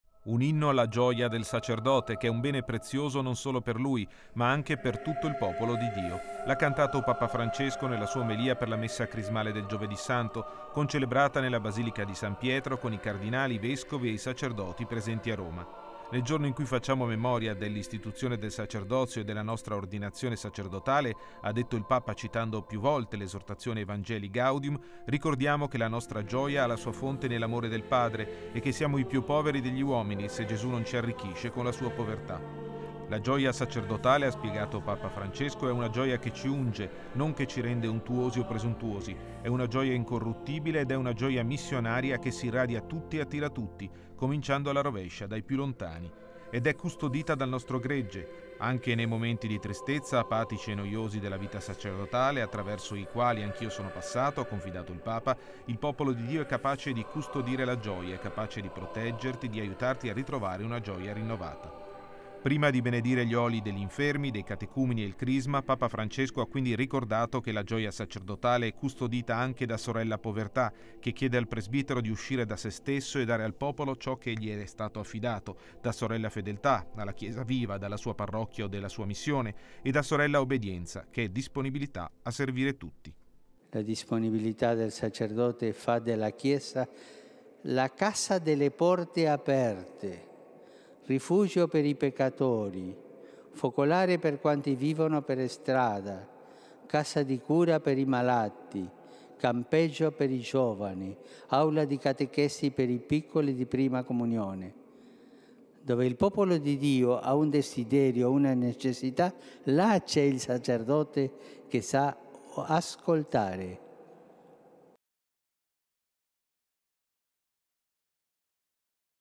Messa Crismale 17-04-2014
Nell'omelia della messa crismale, prima della benedizione degli olii, Papa Francesco ha ricordato che la gioia sacerdotale è missionaria e legata alla povertà, alla capacità di uscire da se stessi per donarsi al popolo di Dio.